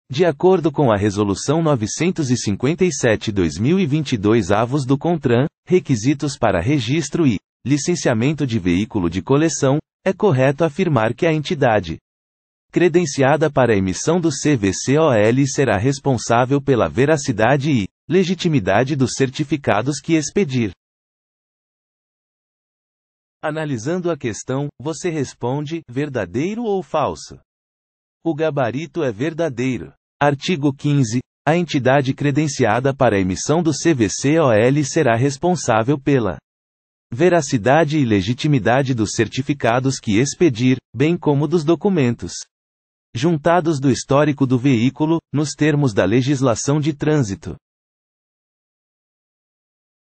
QScon Direito: [Art. 15, Res 957/2022, CONTRAN] Texto de Lei
Art. 15, Res 957/2022, CONTRAN | Texto de Lei

De acordo com a Resolução 957/2022 do CONTRAN (Requisitos para registro e. licenciamento de veículo de coleção), é correto afirmar que a entidade. credenciada para emissão do CVCOL será responsável pela veracidade e. legitimidade dos certificados que expedir.

Gabarito: Verdadeiro